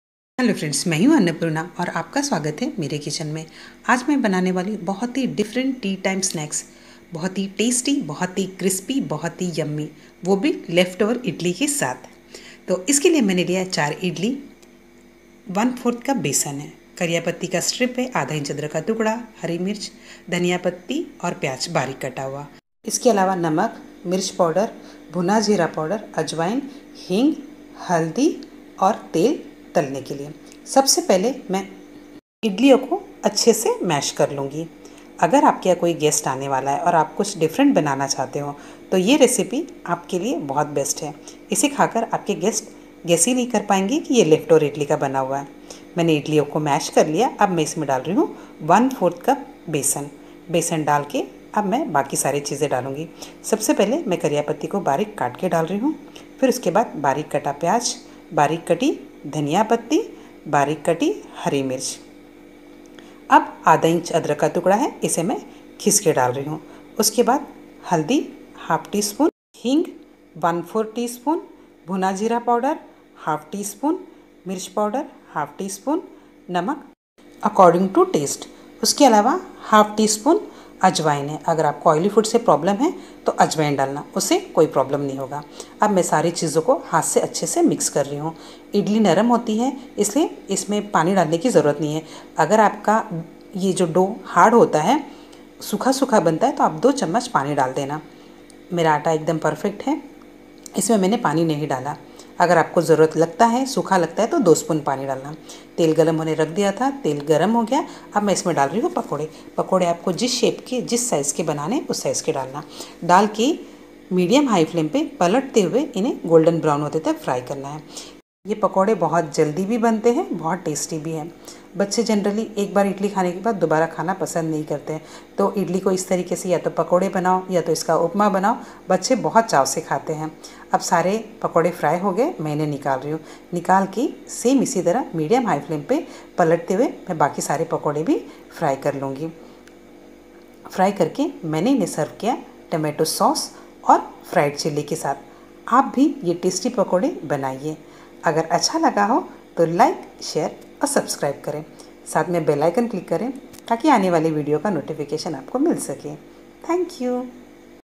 बचे हुए इडली के कुरकुरे पकोड़े Idli Pakoda.Crispy Pakora With Leftover Idli.Best Use Of Leftover Idli
Hello friends Today I will share Idli Pakoda.Crispy Pakora With Leftover Idli.
I hope you will like this recipe.
 Ingredients
4 - Left over Idli batter
1/4 cup - Besan
1/2 tsp - Cumin  powder
1/2 tsp - Chilli powder
1/2 tsp - turmeric/haldi
1/4 tsp hing
1/2 tsp ajwain/ carom seeds
Salt  to taste
1-Onions (finely cut)
2- Green Chilli (finely cut)
1/4 cup - Coriander leaves (finely cut)
1 strip - curry leaves
Oil for frying
Dear friends ,If you like this video then ,

हरी चटनी बनाने का नया तरीका. Instant Mint /Pudina Chutney For Any Snacks Or Dhokla. पुदीना चटनी

friends ,If you like this video then ,